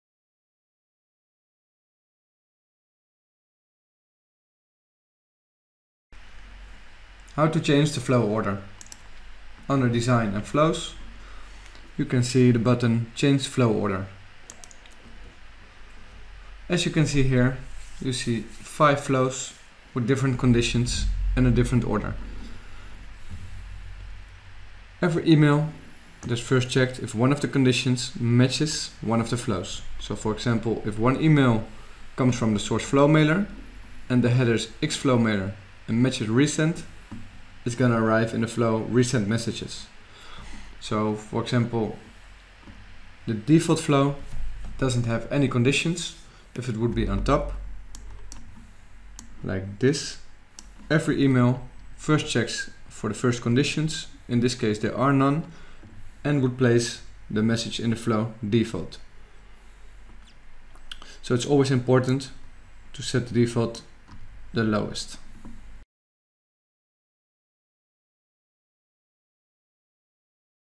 Design: change flow order | Flowmailer Academy
Learn how to change your rules and priorities with  this Flowmailer Academy Tutorial.